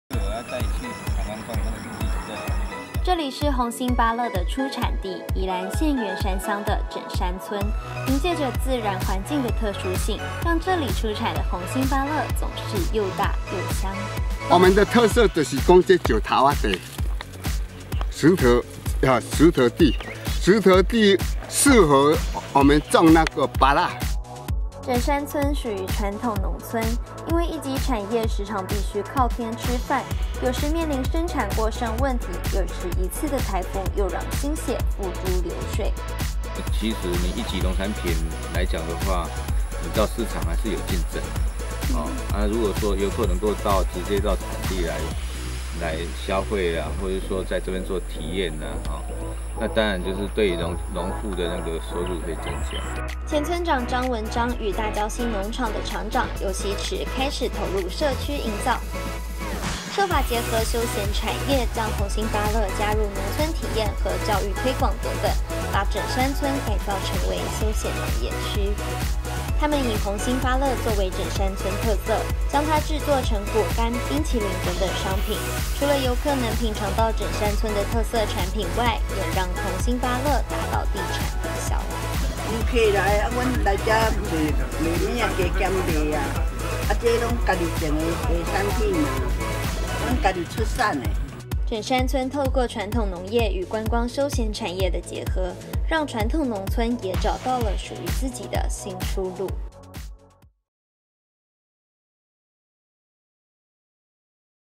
紅心芭樂故鄉 枕山村的農業升級
【記者陳思錡、王傳豪／宜蘭縣報導】紅心芭樂因為口感軟Q，果肉裡帶有香氣，深受喜愛，在市場上時常能看到它的身影。而宜蘭縣員山鄉的枕山村得天獨厚的自然環境，造就了紅心芭樂的盛產，枕山村將農產品結合觀光產業，也研發出相關食品，例如冰淇淋、果乾等等，讓遊客一邊認識紅心芭樂，也一邊暢遊於它的故鄉——枕山村。